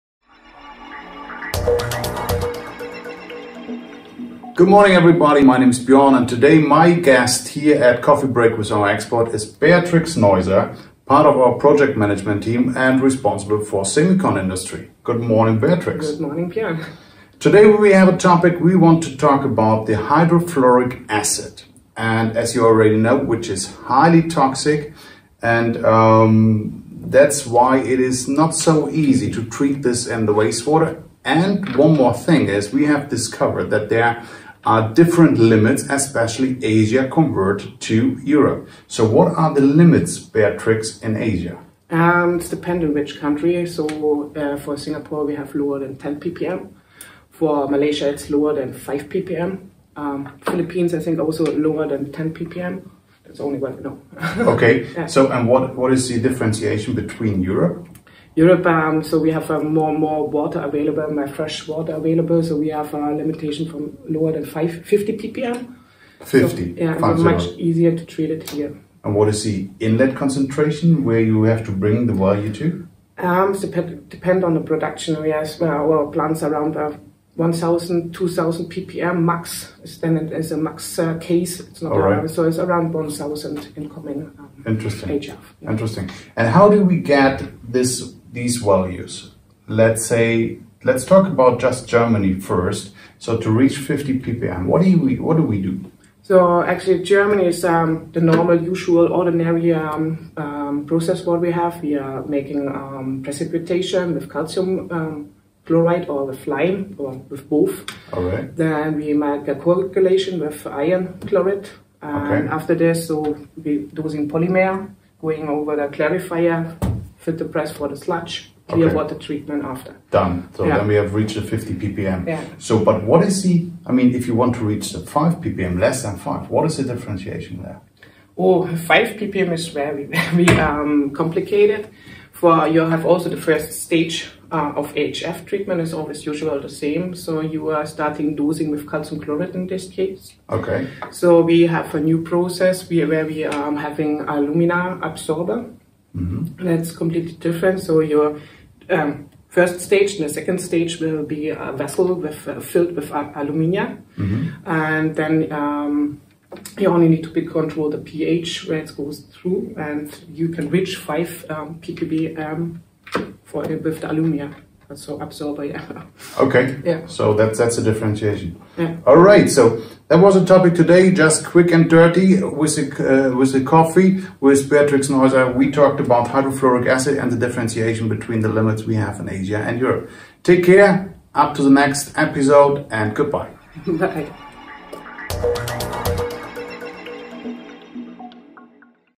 Hydrofluoric Acid treatment from Semiconductor Production
Large quantities of hydrofluoric acid (HF) are required for the etching processes during production and are later found in the wastewater. Before wastewater can be discharged or reused, hydrofluoric acid and its salts must be removed. The wastewater is treated by precipitation and flocculation. The fluoride contained is precipitated as calcium fluoride by the addition of lime milk or calcium chloride.